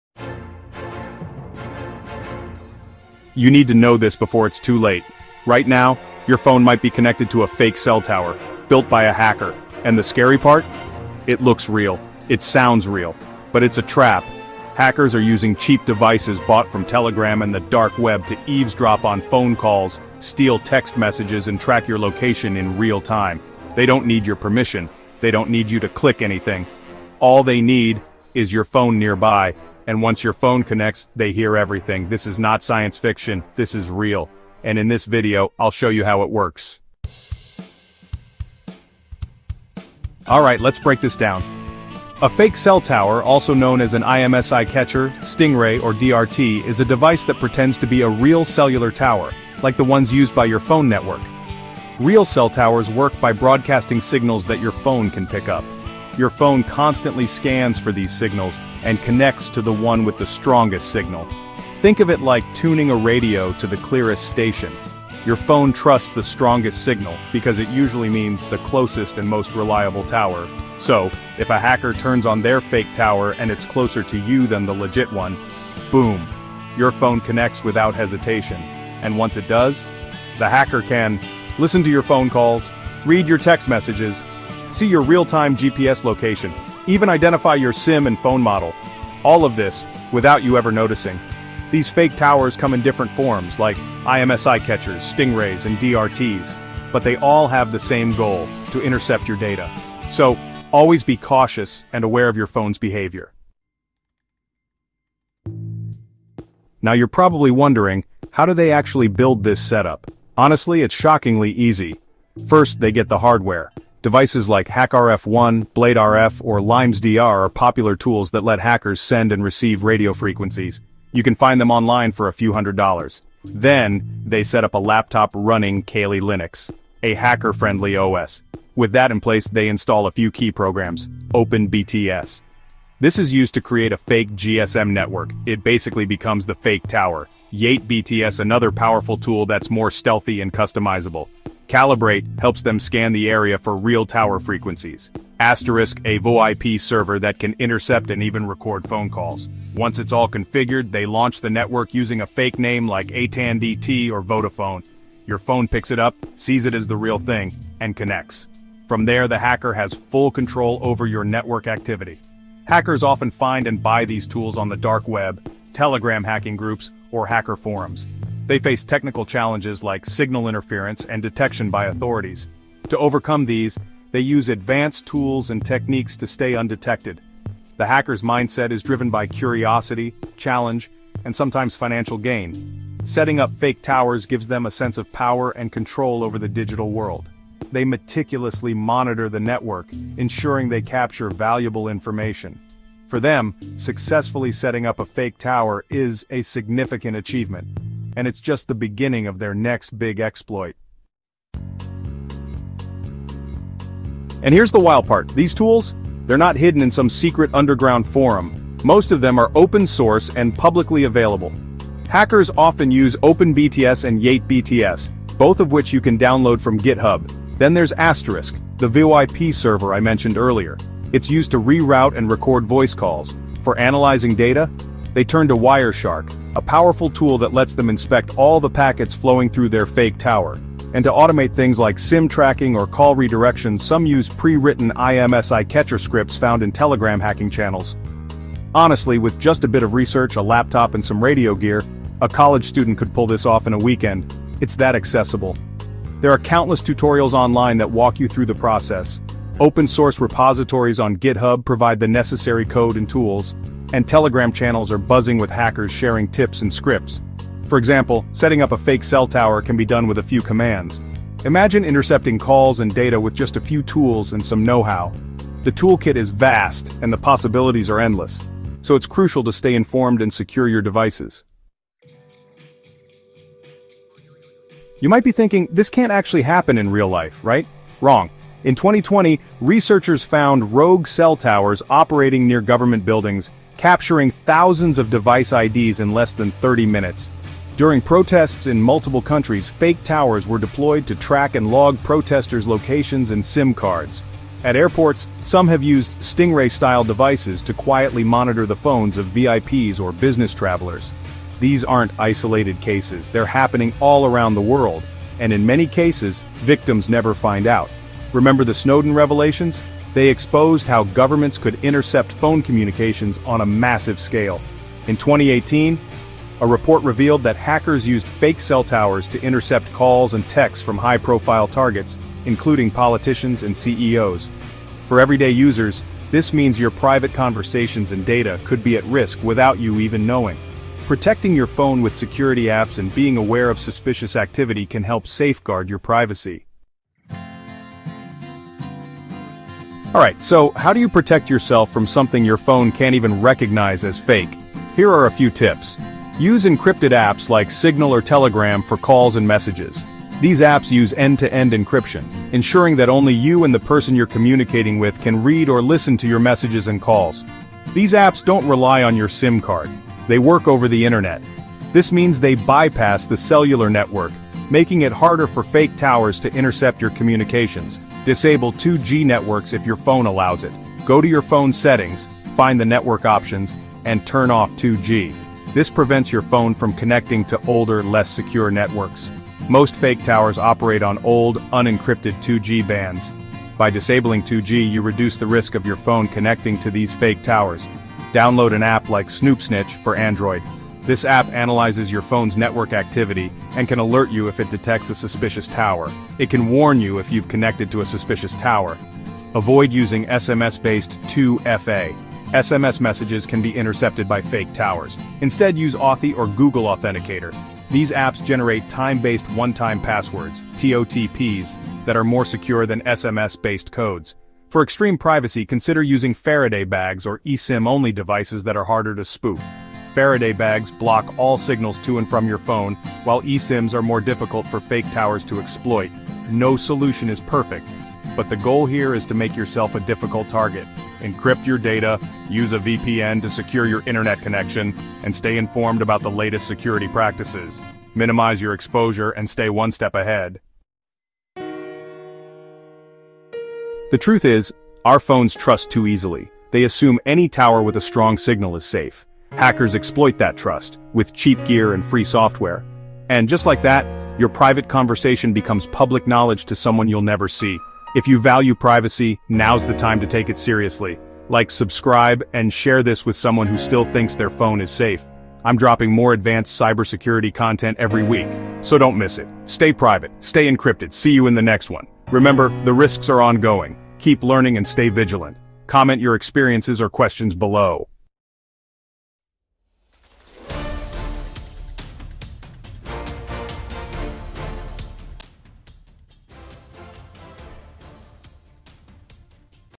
How Hackers Secretly Listen to Your Phone Calls using a Fake Cell Tower!
🚨 How Hackers Spy on Your Phone Calls Remotely – Without Touching Your Phone! 🚨

Did you know hackers can perform a fake cell tower attack—also called an IMSI catcher or Stingray device—to carry out phone call hacking and call interception without ever touching your phone? This terrifying exploit is possible through vulnerabilities like SS7 exploits and VoIP hijacking, allowing cybercriminals and intelligence agencies to eavesdrop on your conversations, read your texts, and even bypass 2FA. In this video, we dive into the world of mobile hacking, showing you exactly how hackers use tools like OpenBTS, YateBTS, and Asterisk VoIP to build rogue towers, where they source hacking tools from the dark web and Telegram hacking groups, and most importantly—how to protect your mobile privacy from these telecom hacking threats.

🔍 How Do Hackers Secretly Listen to Your Calls?

Hackers set up a fake cell tower (IMSI catcher), forcing your phone to connect. Once hooked, they exploit:

OpenBTS & YateBTS (open-source tools on GitHub) for creating a rogue GSM network

SS7 exploit techniques to manipulate global mobile networks and reroute calls

Asterisk VoIP servers to intercept and record voice calls in real time

Wireshark for packet analysis and VoIP hijacking to capture internet-based calls

SDR hacking hardware like HackRF One or BladeRF bought via dark web tools or Telegram channels

IMSI Catcher Scripts that automate network spying and SIM tracking

They combine these hacker hardware and hacking techniques—from Pentesting tools to cell tower vulnerability exploits—to eavesdrop on your every word.

📺 OUTLINE:

00:00:00 Cell Tower Spoofing Attack Explained
00:00:36 What Is a Fake Cell Tower?
00:01:43 How Hackers Set It Up
00:03:20 Tools Hackers Use
00:04:31 Real-World Incidents
00:05:36 How to Protect Yourself
00:07:20 Final Thoughts

🛡 How to Protect Yourself from Fake Cell Tower & SS7 Attacks:

✅ Use Encrypted Call Apps – Switch to Signal, WhatsApp, or Telegram for end-to-end encrypted voice and text
✅ Disable 2G Connections – Prevent fallback to insecure networks that enable SS7 hacking
✅ Avoid Public Wi-Fi for VoIP Calls – Hackers can perform MITM attacks and VoIP hijacking on unsecured networks
✅ Keep Your Phone Updated – Security patches help defend against emerging mobile hacking and cybercrime exploits
✅ Install Anti-IMSI Catcher Apps – Use SnoopSnitch or CellSpyCatch to detect suspicious towers and network spying attempts
✅ Use a VPN to Encrypt Internet Traffic – Protects against MITM attacks, VoIP hijacking, and other network vulnerabilities

Stay alert, stay encrypted, and stay one step ahead of the hackers!